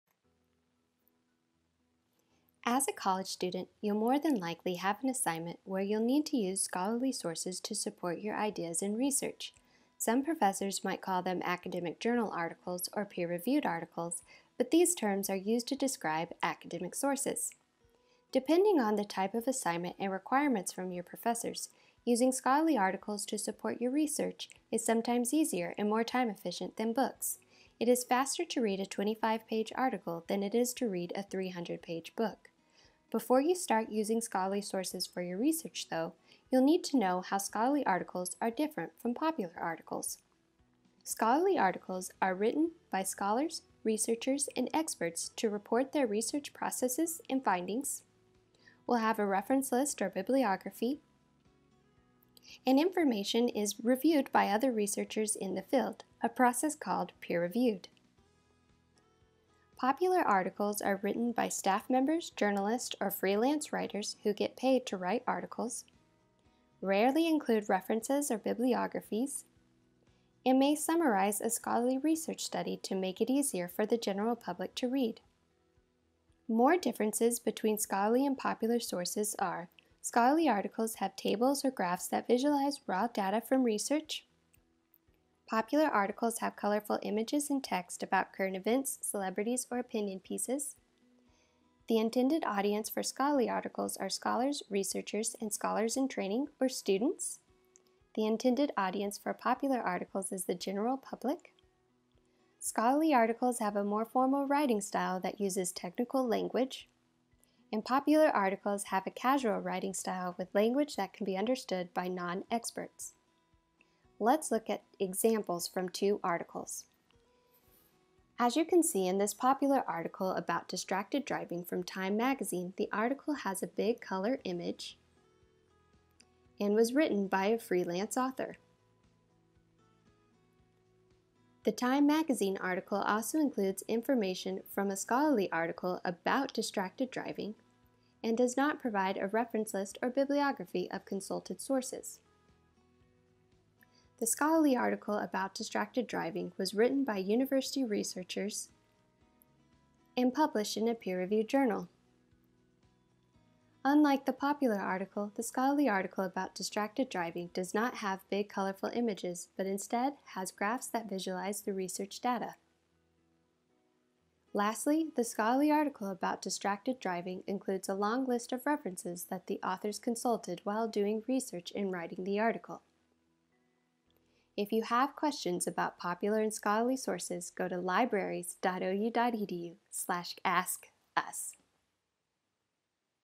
Popular and Scholarly Sources
Learn more about scholarly sources, their differences from popular articles, and how to distinguish between the two.

Have questions? Visit libraries.ou.edu/askus